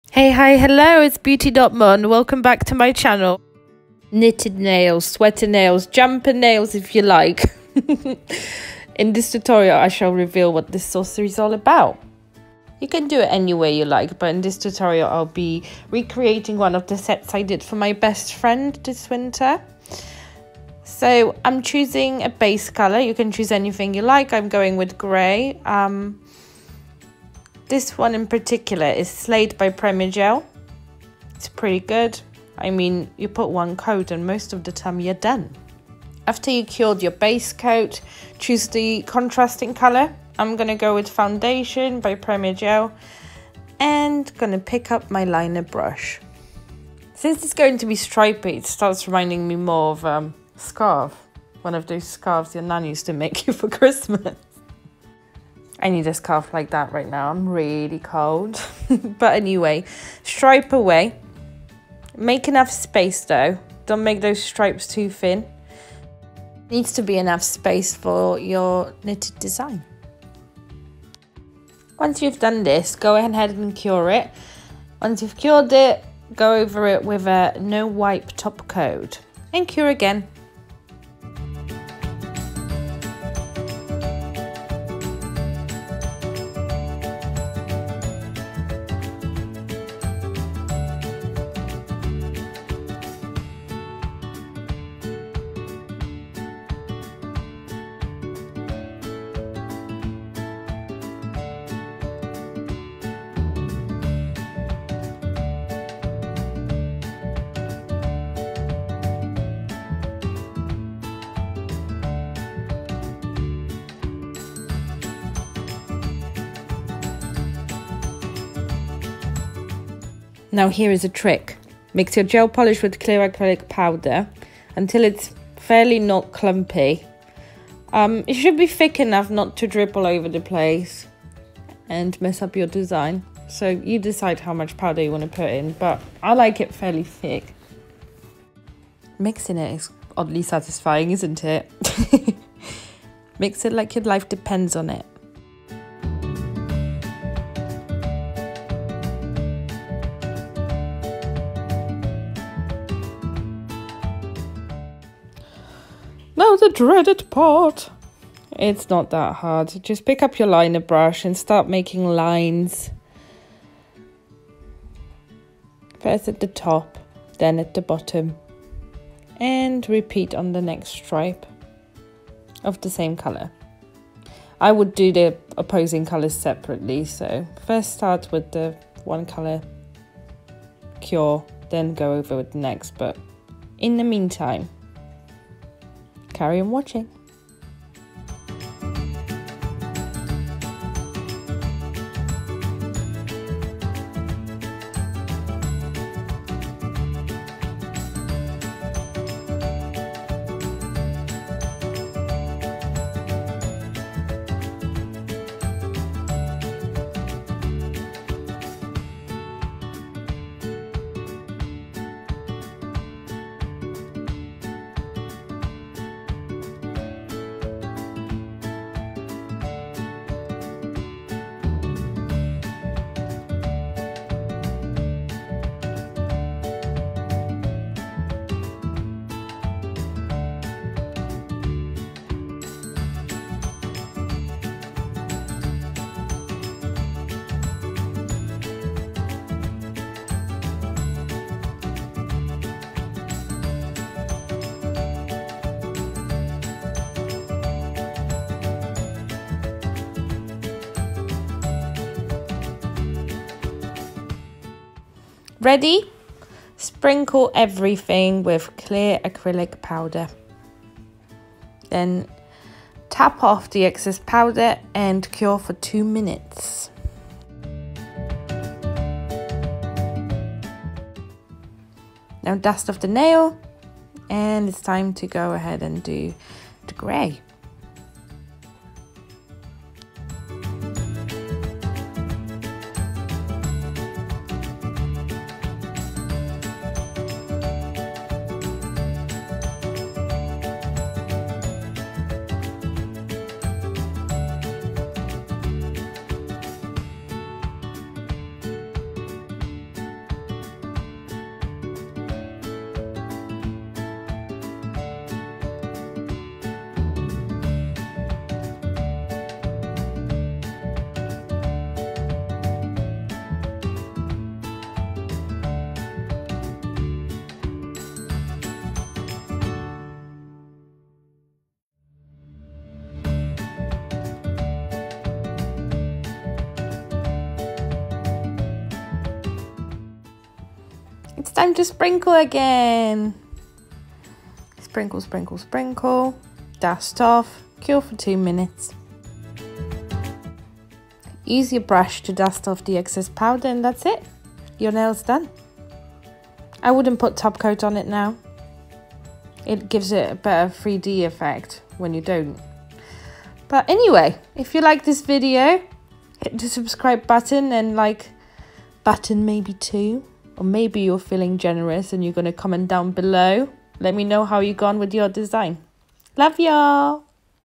HOW TO: Easy Knitted Sweater Nails - Nail Art Tutorial
In this video you'll learn how to create jumper nails. Knitted designs have been around for a good few years now, but a lot of Nail Techs dread the idea of trying it out. I know, because I was one of them.

I'll be using a mixture of Gel Polish and clear Acrylic Powder to achieve this look. Join me and let me know how you got on in the comments.

If you like my videos, hit the subscription and like button :)

Thank you all so much for watching, I hope you enjoyed it!